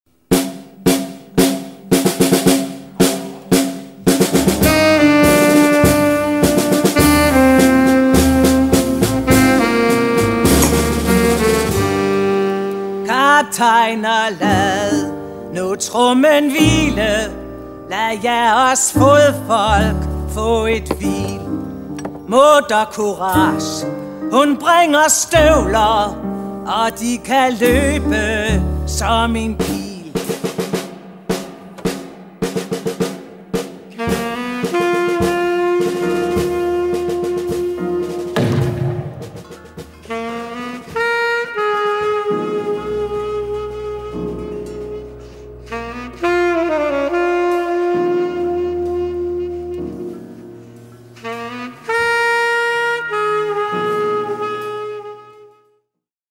Mutter Courage
Oplev Karen-Lise Mynster i en af verdensdramatikkens mest
ikoniske kvindeskikkelser som Mutter Courage. Bertolt Brechts
anti-krigsdrama er en tidløs og rystende demonstration af, hvordan mennesket ikke
formår at lære af dets lidelse. Courage fodrer fronten med snaps og
støvler, men hun må snart sande, at krigen æder alt hvad hun har kært.